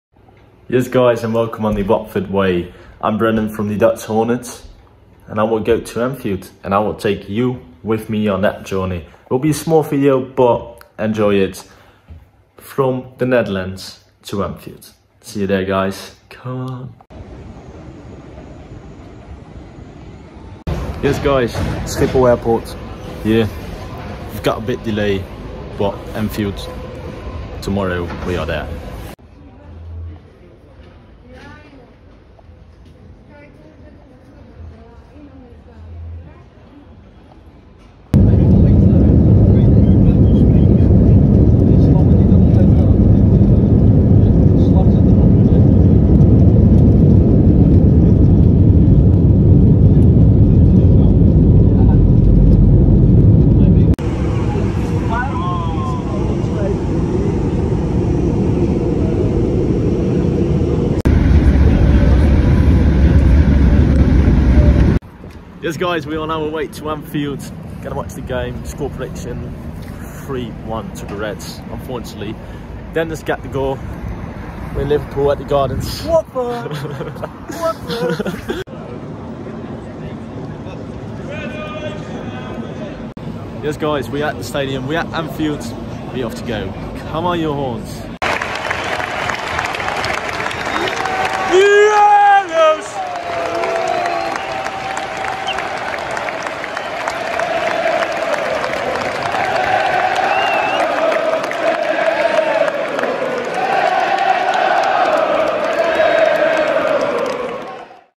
Liverpool 2-0 Watford | Travel Vlog
Liverpool beat Watford 2-0 at Anfield in the Premier League with Diogo Jota and Fabinho on the scoresheet.

Join Brendan as he takes you on his journey from the Netherlands to Liverpool.